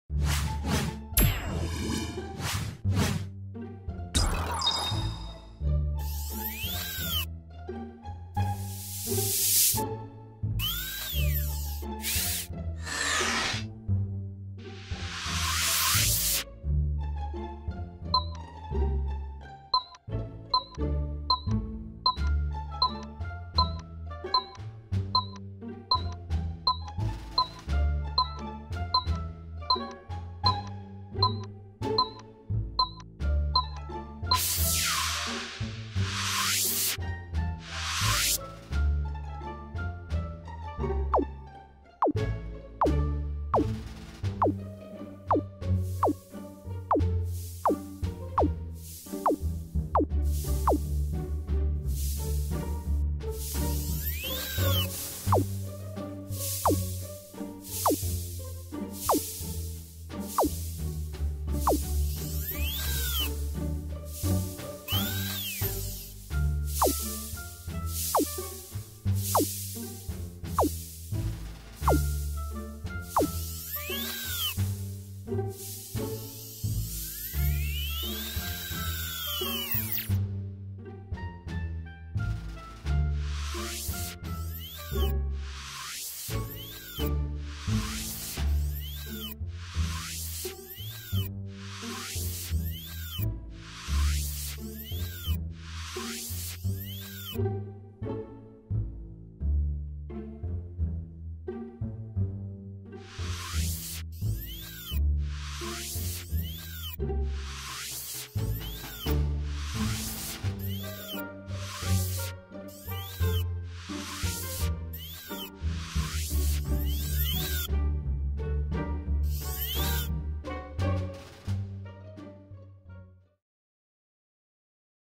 VIDS for KIDS in 3d (HD) - Jumping fun with Hairy Balls - AApV
Hey it´s just hairy balls hypnotizing and entertaining cartoon for toddlers. Cute and simple... Whatever you do.. just keep jumping !